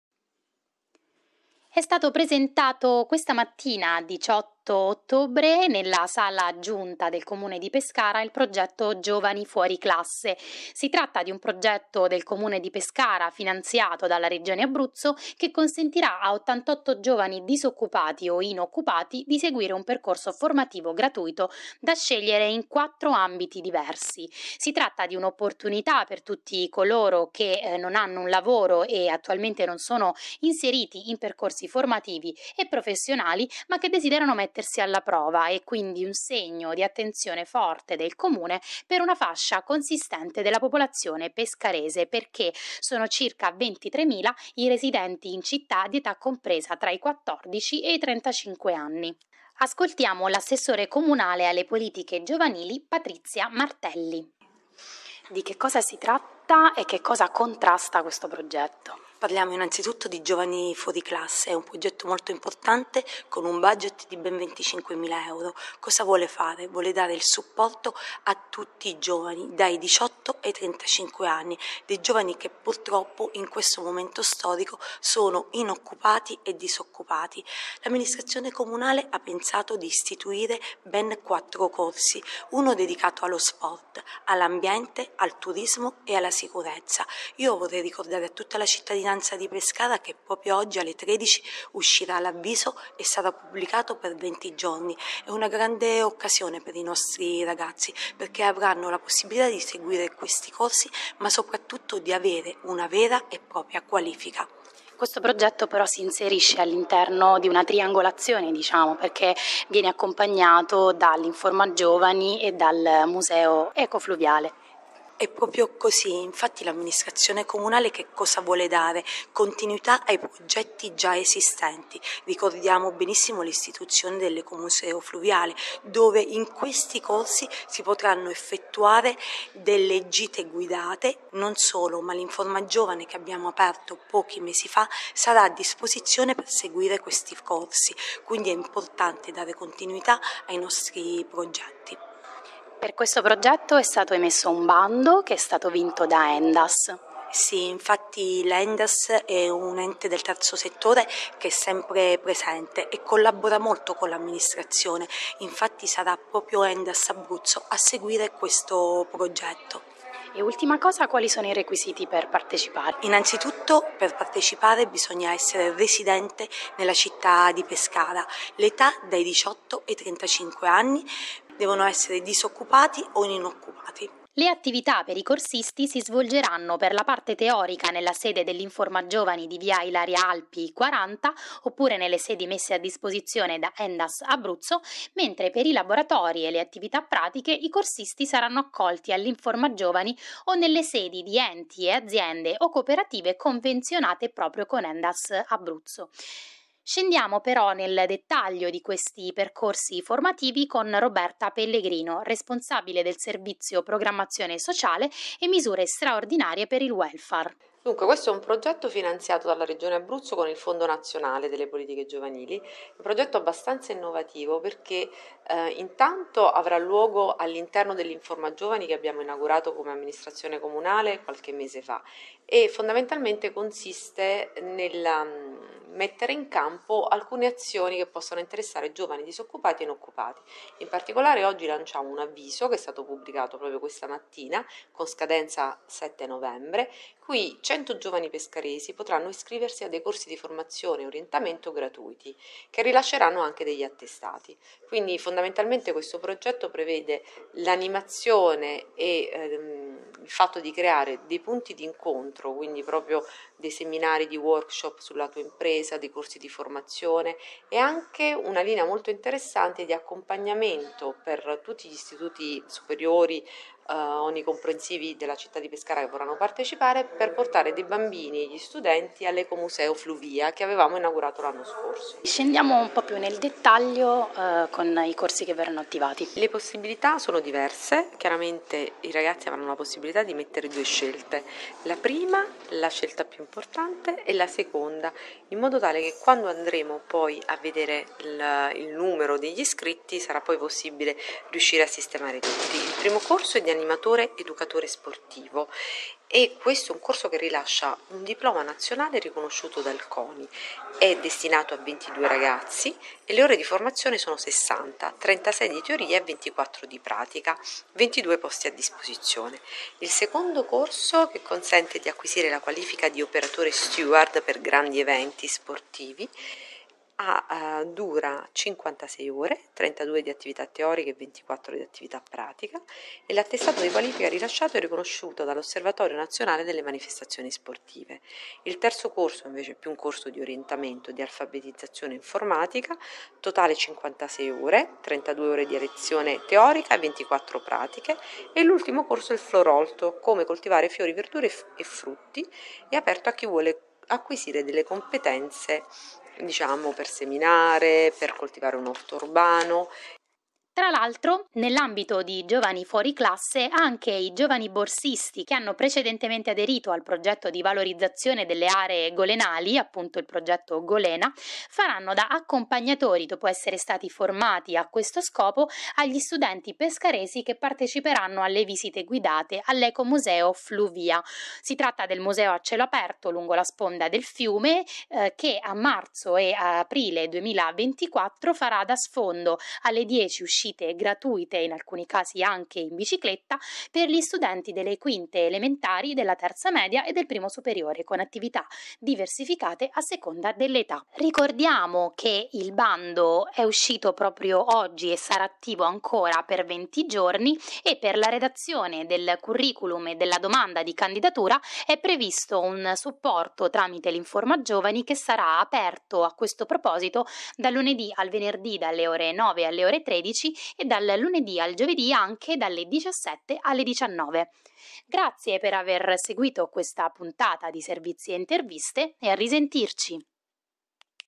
Servizi e interviste - "Giovani fuoriclasse: contrastiamo l'inoccupabilità"
Il Comune di Pescara mette a disposizione quattro corsi di formazione gratuiti dedicati a inoccupati e disoccupati, per aiutare i cittadini residenti in città entro i 35 anni a trovare uno sbocco lavorativo. Nel servizio tutti i dettagli su posti, scadenze e attività.